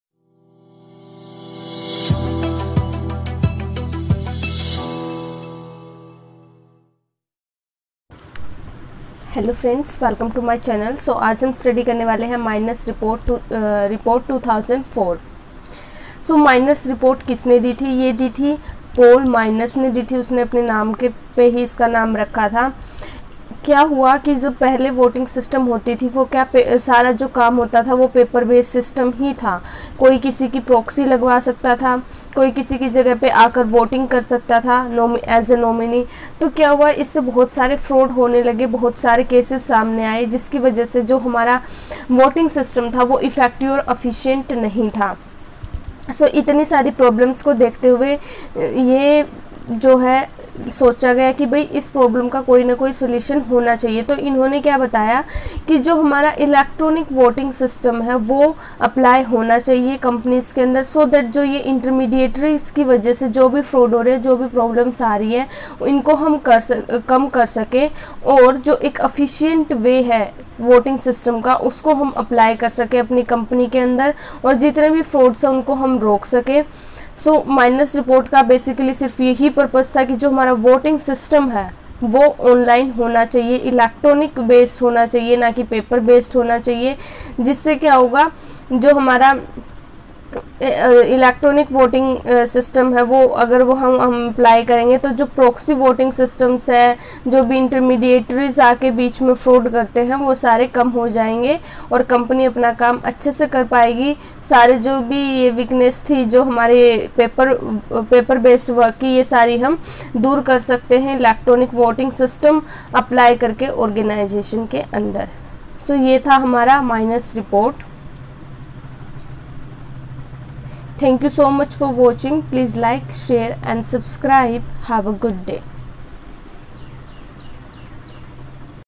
#26. Myners Report (2004) on Corporate Governance in International Context in Hindi & English
#25. Myners Report (2004)  on Corporate Governance in International Context in Hindi & English

Myners Report (2004) in Hindi & English

The report

Institutional investment in the United Kingdom: a review
Review published on the HM Treasury website in March 2001 (now hosted by National Archives). The report was announced in the HM Treasury press release 'Myners Review sets out proposals for new approach to institutional investment' on 6th March 2001

Voting improvements: Myners report

Paul Myners has reviewed the impediments to voting UK shares for the benefit of the Shareholder Voting Working Group. He recommends that all participants in the voting process make certain improvements.

Paul Myners, the former fund manager who reported on institutional investment in 2001, has reviewed the impediments to voting UK shares for the benefit of the Shareholder Voting Working Group (SVWG). His report contains an analysis of the voting system, which is complex, involves a large number of participants and, for these reasons, proves faulty on occasions. Proxy votes by institutional investors sometimes get lost before they reach the registrar; a problem that faced some Unilever shareholders in 2003.

Myners recommends that all participants in the voting process make certain improvements. He is strongly in favour of electronic voting, because of the enhanced efficiency it would bring, and encourages issuers in at least FTSE 350 companies to introduce system changes to allow for it as soon as practicable.

The view of Robert Blanks, Deputy Director of the Policy Unit, Institute of Chartered Secretaries and Administrators (ICSA), is that most FTSE 100 companies have already provided for electronic voting in their articles of association. (ICSA is one of the associations represented on the SVWG.) The next step will be getting buy-in from all participants.

Another recommendation from Myners is that issuers should call a poll on all resolutions at company meetings. This is different from the line taken by ICSA, although its guidance is for companies of all sizes. Myners is basically addressing FTSE 350 companies and these larger bodies are more likely to call a poll as a matter of routine.

Thankuu for watching
kurukshetra University, kurukshetra
distance education

Thankuuu soo much for watching 🙏.

Have a wonderful day 😊 keep supporting me.